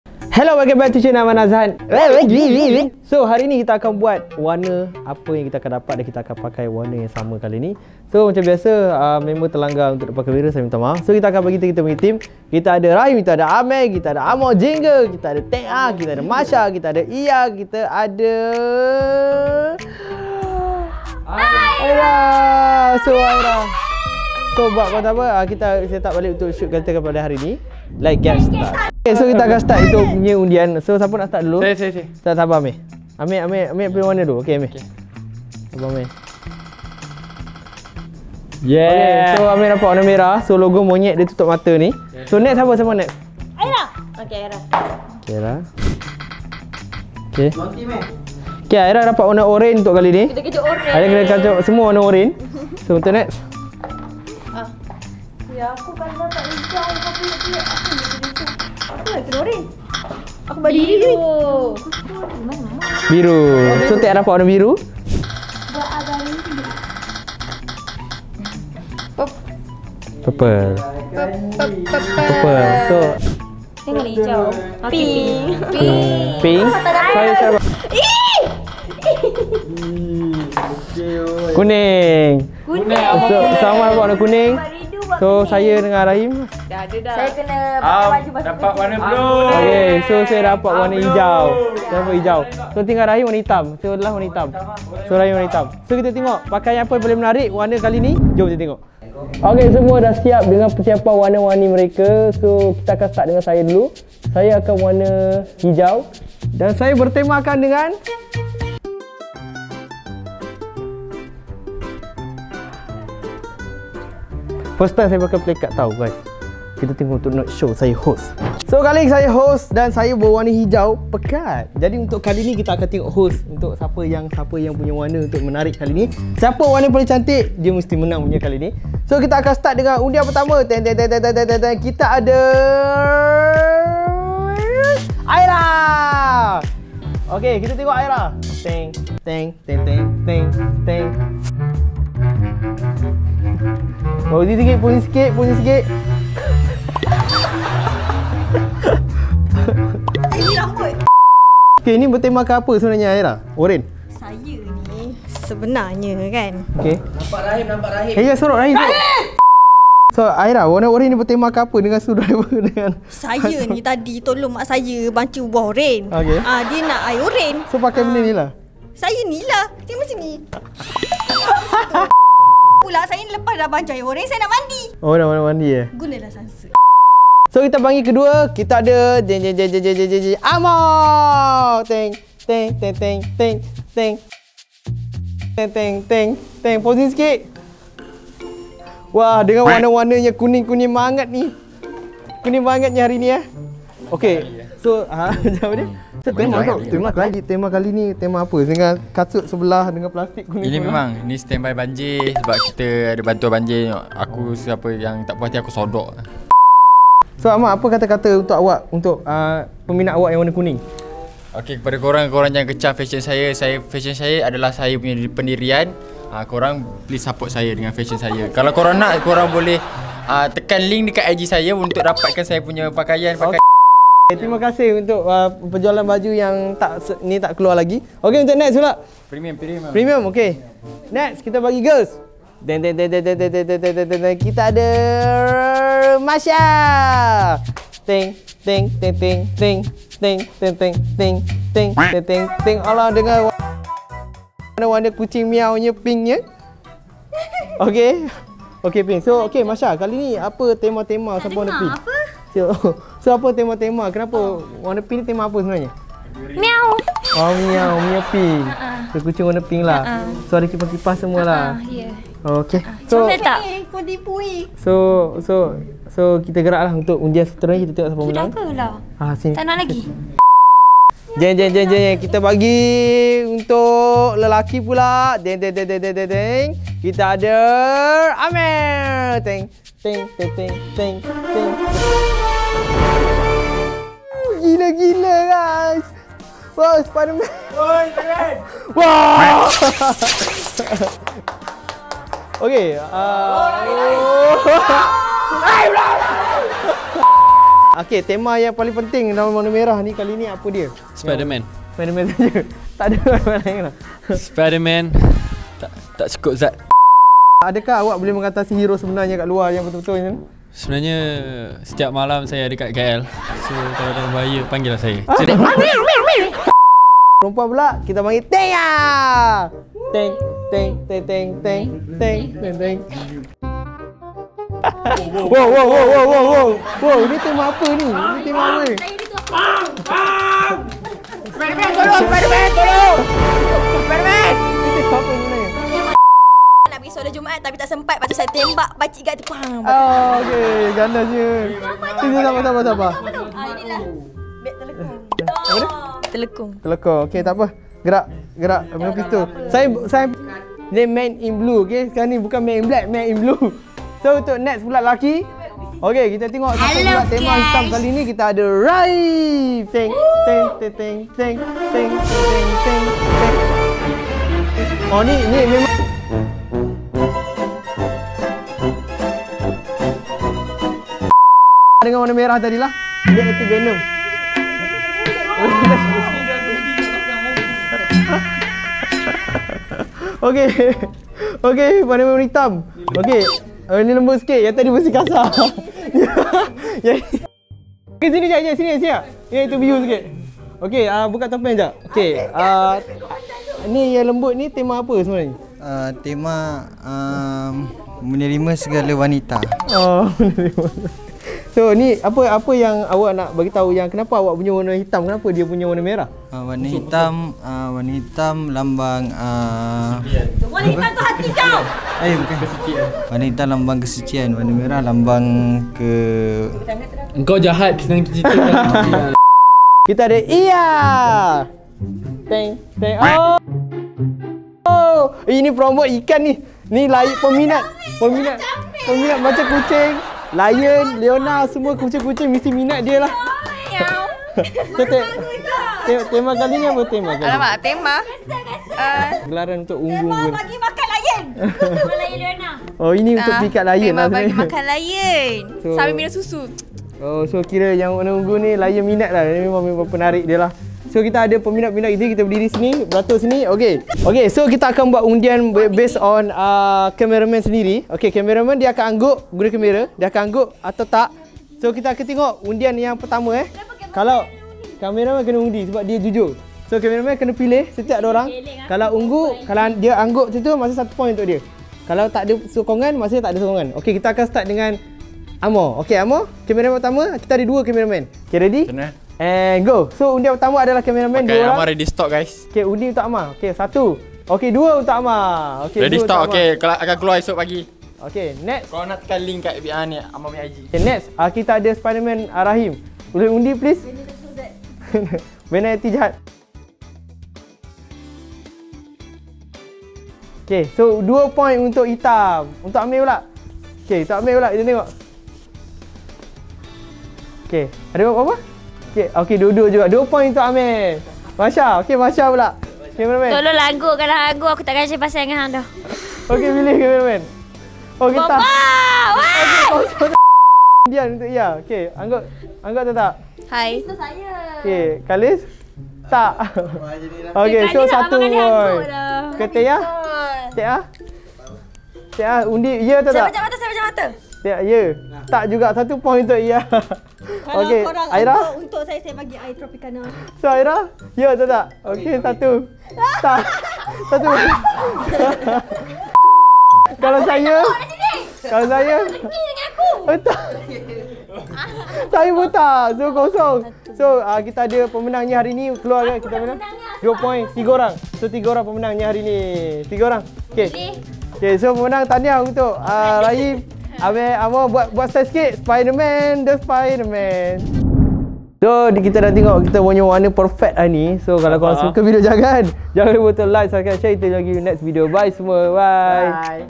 AMIR & RAHIM JADI SPIDERMAN !!- PAKAI BAJU IKUT WARNA !!
Jom kita tengok pakaian siapa paling menarik.